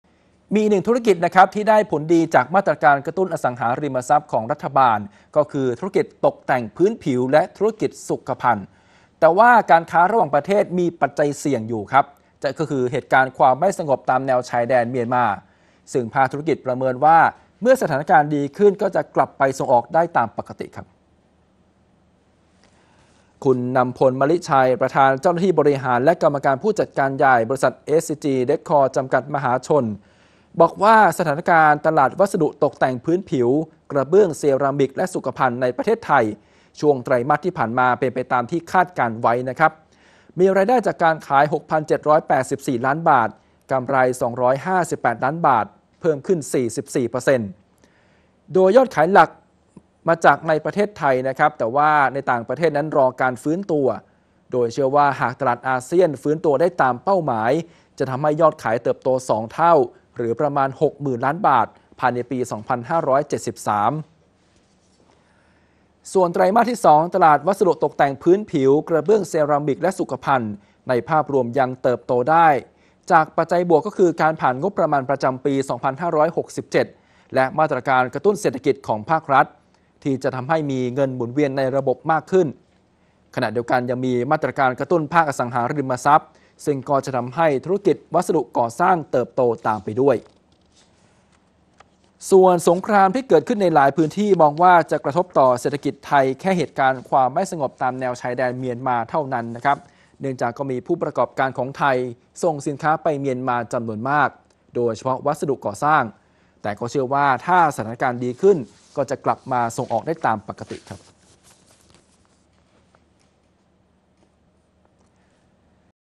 ภาคธุรกิจเชื่อเหตุการณ์ความไม่สงบตามแนวชายแดนไม่กระทบส่งออก ข่าวค่ำ วันที่ 23 เมษายน 2567
อีกหนึ่งธุรกิจที่ได้รับผลดีจากมาตรการกระตุ้นอสังหาริมทรัพย์ของรัฐบาล ก็คือ ธุรกิจตกแต่งพื้นผิวและธุรกิจสุขภัณฑ์ / แต่ขณะนี้ ก็มีปัจจัยเสี่ยงจากเหตุการณ์ความไม่สงบตามแนวชายแดนเมียนมา  /แต่ ภาคธุรกิจ ยังคงเชื่อว่า เมื่อสถานการณ์ดีขึ้น ก็จะกลับไปส่งออกได้ตามปกติ